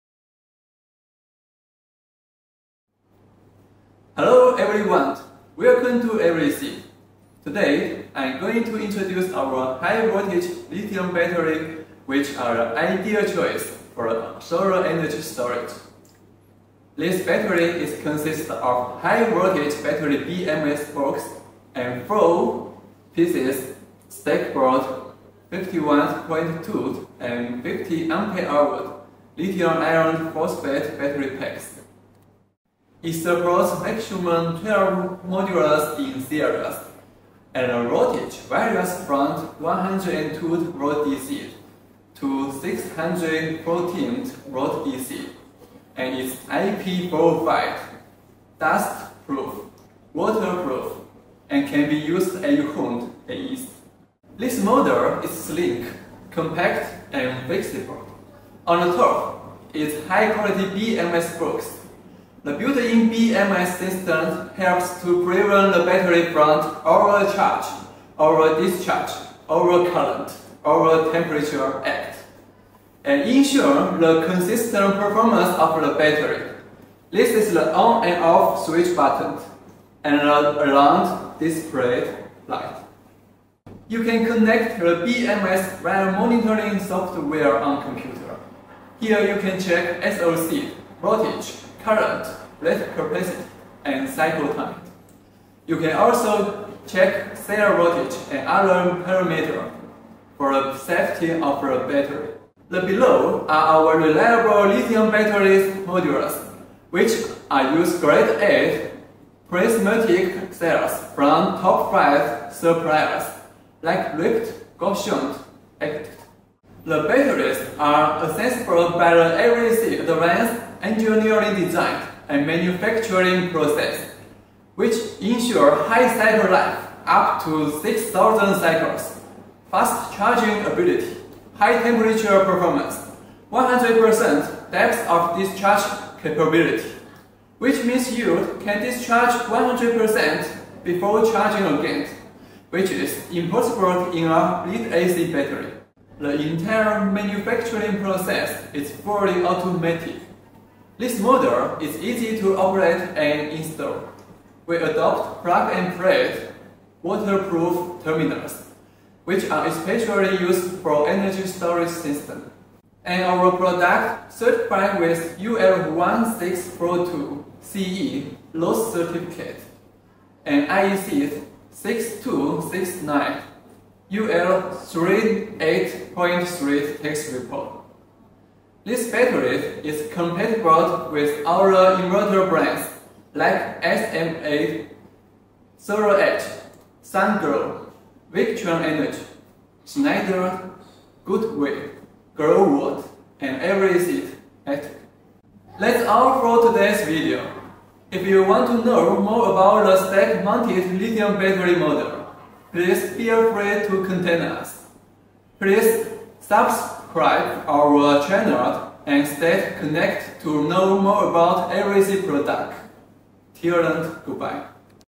High Voltage Stackable energy storage lithium batteries
Based on our batteries technical features of excellent performance on higher temperature, higher capacity range, lighter weight and pollution free operation, our LiFePO4 battery is widely popular by users with a rapid development stance. It is the first choice for the high requirement energy storage battery solution.